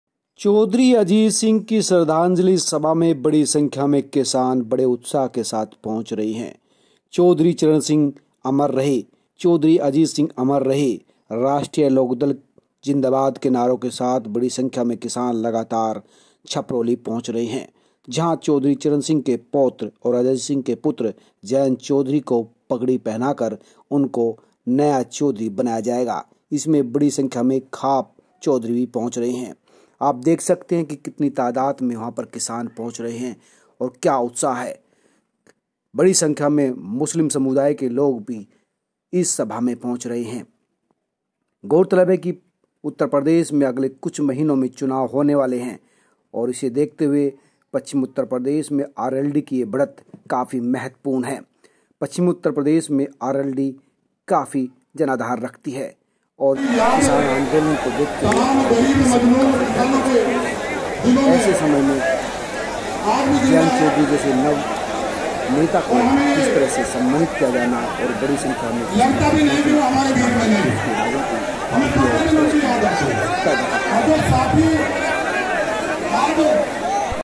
chaproli मेंJayant Chaudhary की पगड़ी सभा और Ajit Singh की श्रद्धांजलि सभा के पंडाल के अंदर का हाल।
Baghpat chhaprauli there is a shradhanjali sabha of Arijit Singh and pagdi program of Jain Chaudhari there is participate former leader and Chaudhari Chaudhary honour to Jayant Chaudhary Jayant Chaudhari is rashtriya lok dal chief and Chaudhary Charan Singh grandson and Ajay Singh son evil new leader of rashtriya lok dal election in 2022 in Uttar Pradesh there is a big political Power is RLS.